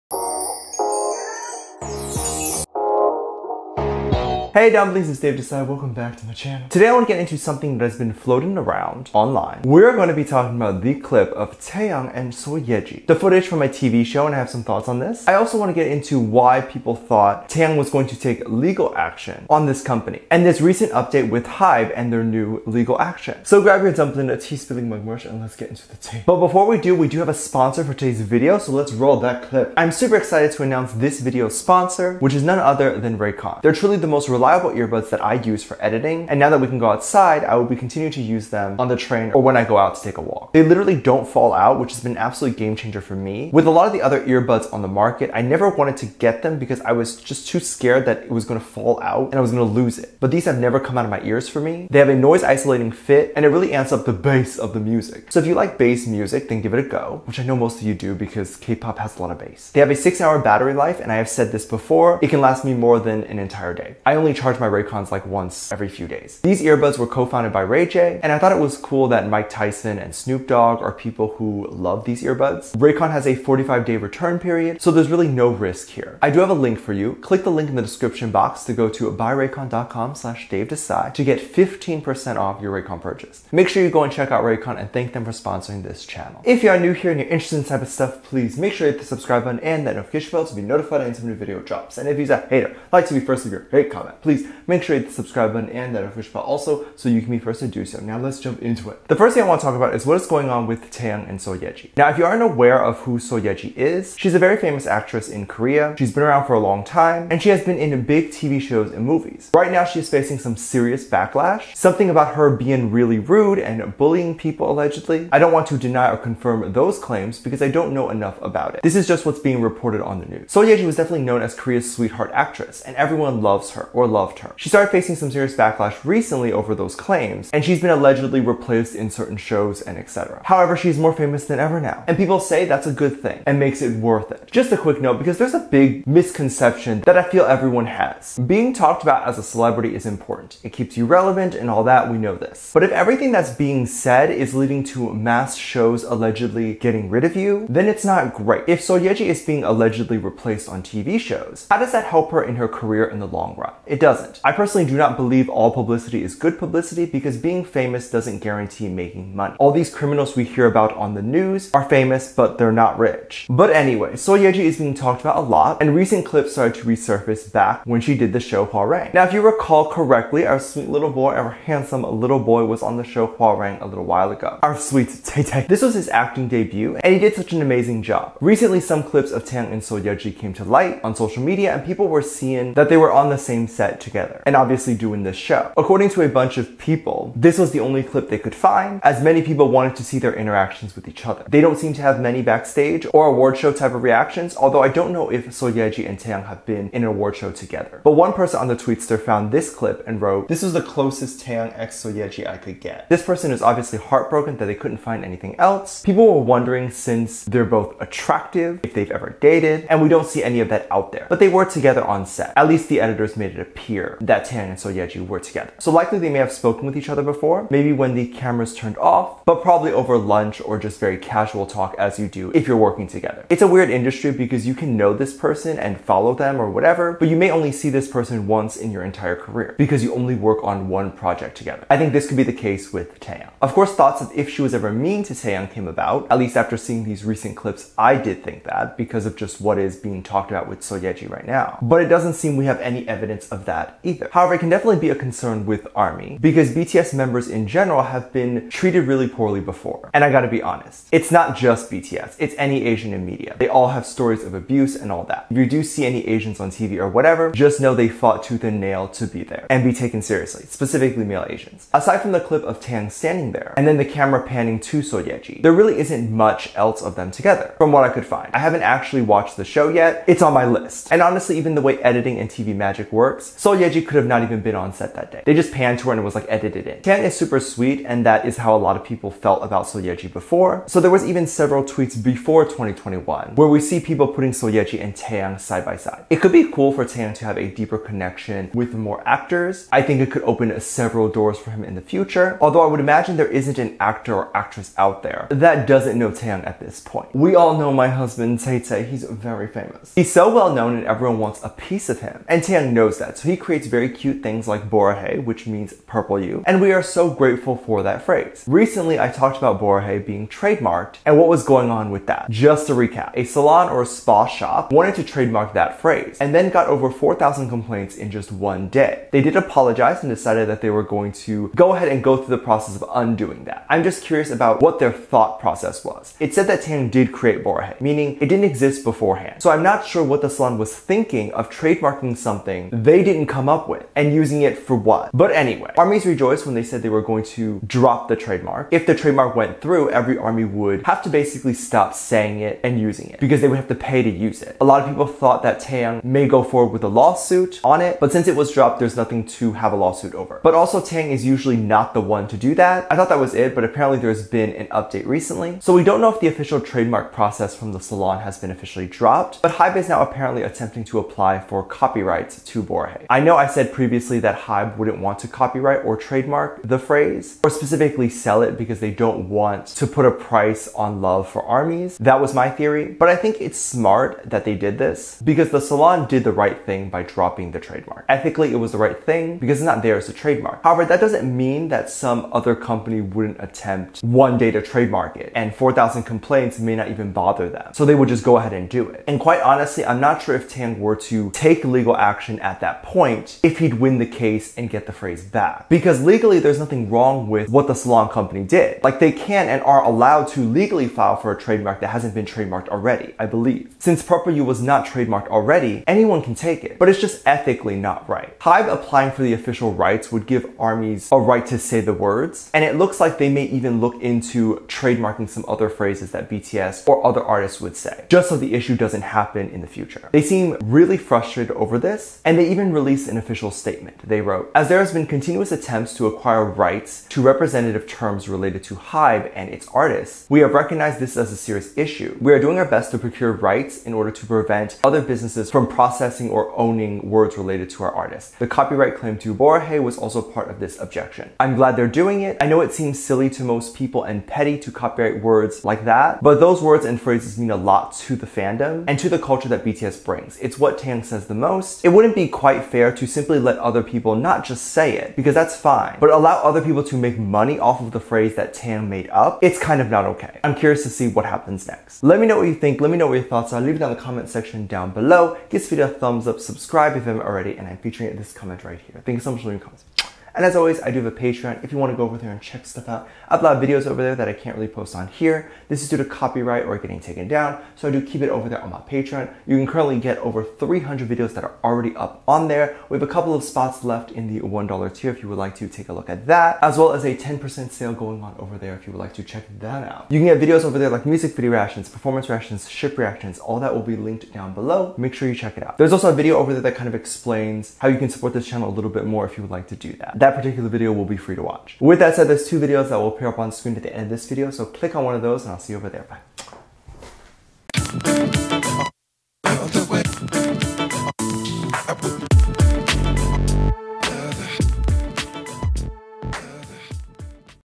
TV SHOW REVEALS Taehyung & Seo Ye Ji Together on Set? V Takes LEGAL ACTION?
Today I wanted to talk about BTS or bangtan sonyeondan boys. Specifically I wanted to get into Taehyung and also an actress named Seo Ye Ji. I wanted to get into what people are talking about online and what people's concerns are. I wanted to get Taehyung's legal action as well and what HYBE might be doing next. Let me know what you think and drop it down below.

LEARN KOREAN w/ 90 Day Korean (I do get a kickback if you sign up using this link)